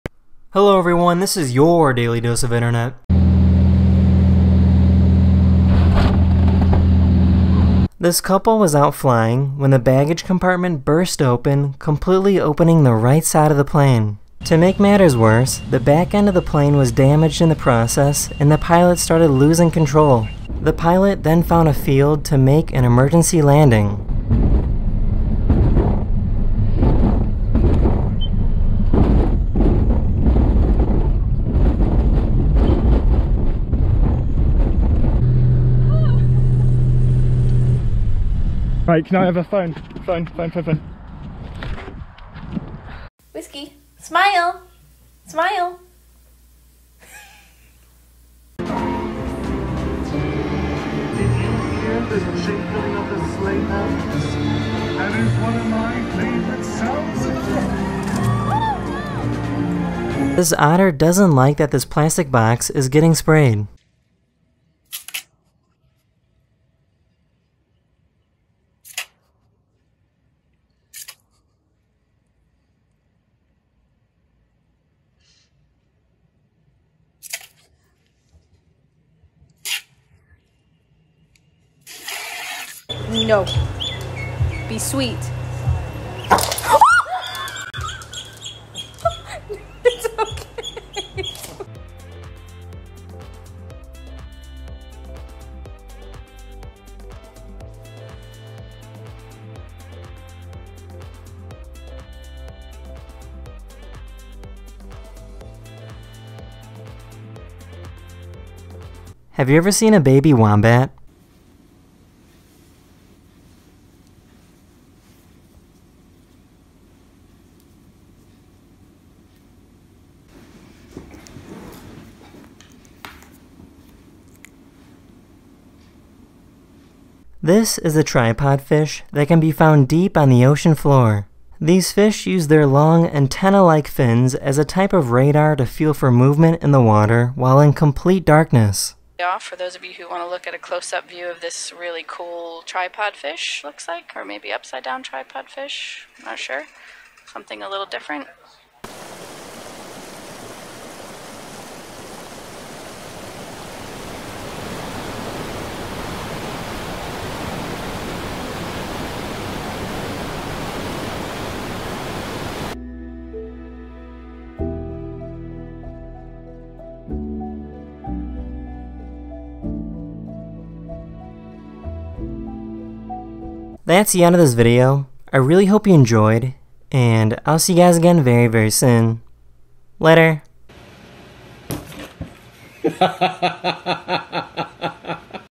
Otter Protects Plastic Box from Garden Hose
Hello everyone, this is YOUR Daily Dose Of Internet! In this video, an otter wants the water to stop coming out of a hose

Puzzle: Ricky L

Also in this video, a dog makes a funny face. Santa falls off his sleigh while in a parade. A cat starts crying from onions. A person base jumps off a cliff. A baby wombat enjoys cuddles. A strange fish at the bottom of the ocean.